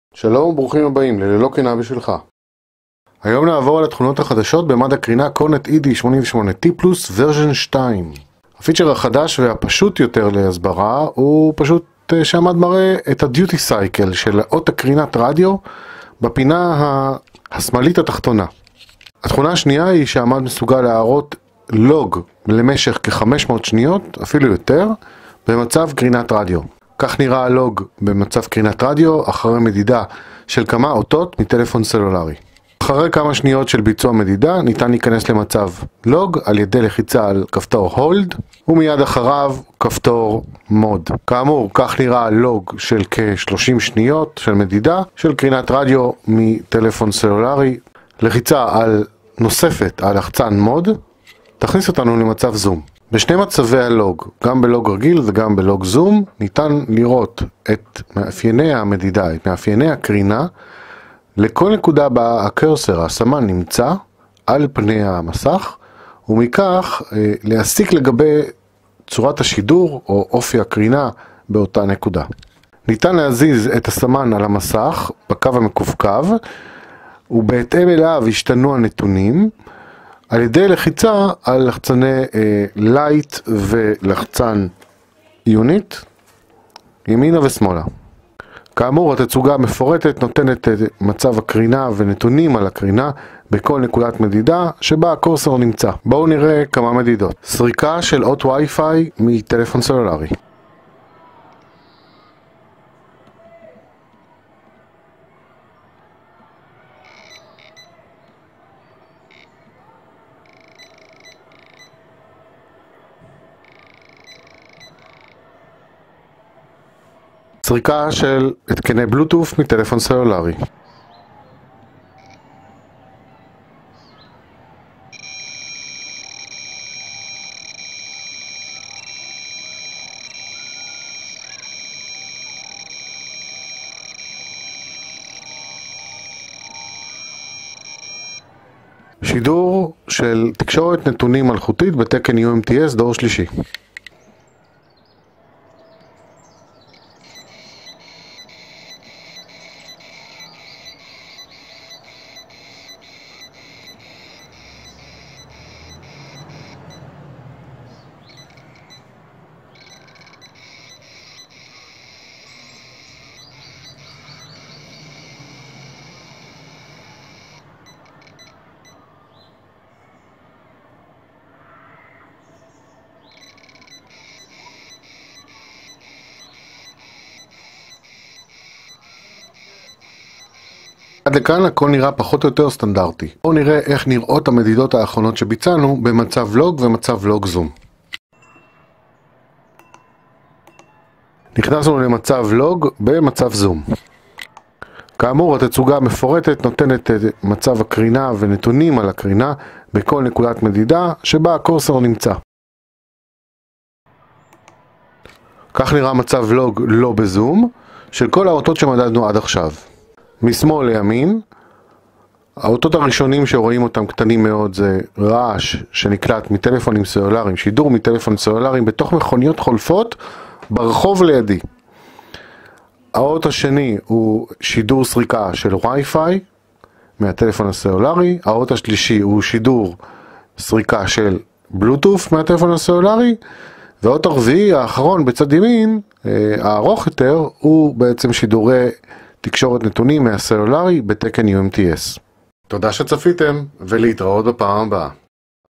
הסברים והדגמה של התכונות החדשות במד הקרינה CORNET ED88TPLUS
הסברים והדגמה של התכונות החדשות במד הקרינה
CORNET ED88TPLUS:
1. DUTY CYCLE באחוזים במצב קרינת רדיו
2. מצב לוג לכ500 שניות במצב קרינת רדיו